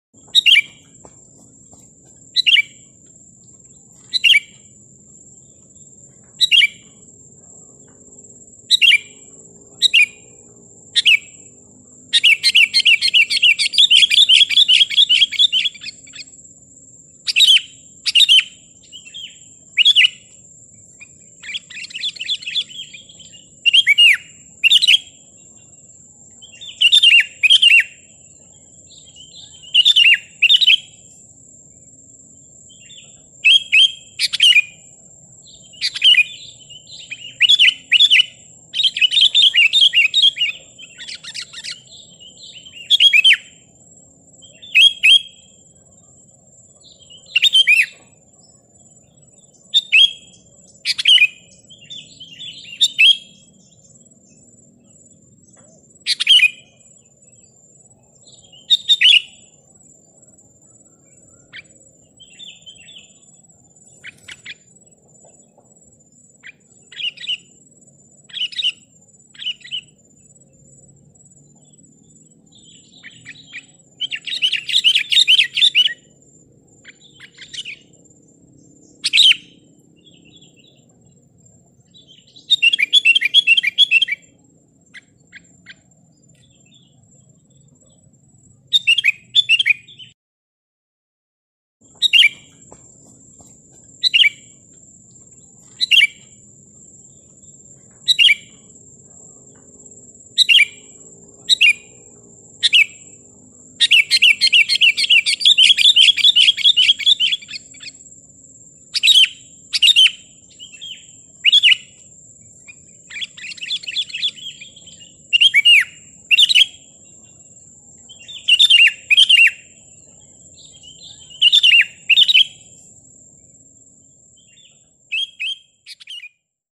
kicau kutilang bikin emosi lawan#masteranburungkutilang#kutilanggacor#suaraburungkutilanggacor
kicau kutilang bikin emosi lawan langsung ngamuk dan gacor buka isian untuk buat kutilang liar akan ikutan emosi setelah mendengar kicau kutilang bikin emosi